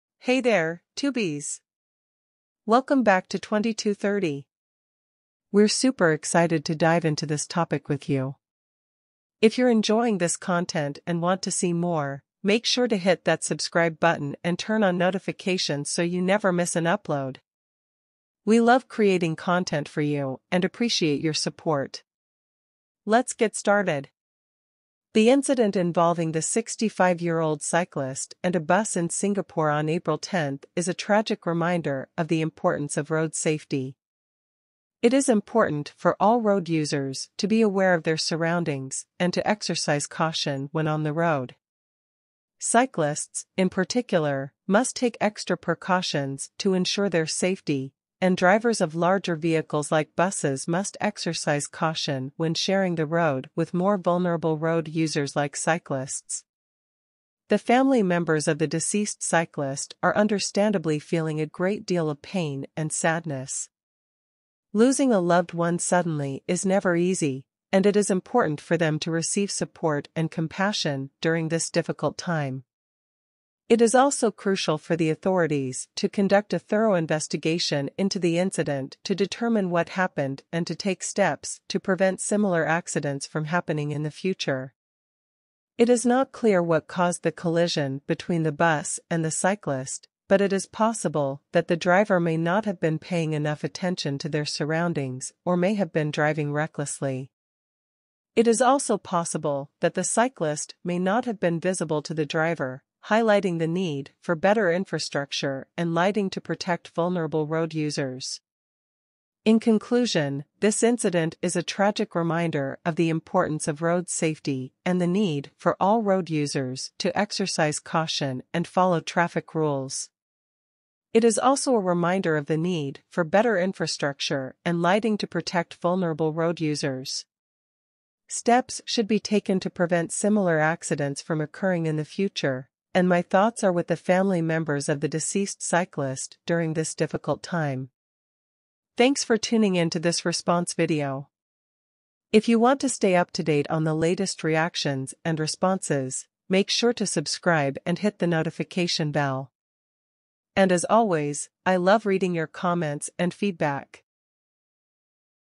Man, 65, dies after bus knocks him down while cycling in Pasir Ris
The tragic incident involving a 65-year-old cyclist and a bus in Singapore on Apr. 10. The man, surnamed Chen, passed away in the hospital after the collision, and his family is finding his sudden passing difficult to accept. The bus driver is assisting with police investigations, and the police are still investigating the incident.

Go-Ahead Singapore, the bus company involved, expressed their condolences and said they are in touch with the victim's family to provide support and assistance during this difficult time. The company also stated that they are cooperating fully with the relevant authorities in their investigations.

It is always a tragedy when a life is lost due to a traffic accident, and my thoughts are with Mr. Chen's family and loved ones during this difficult time.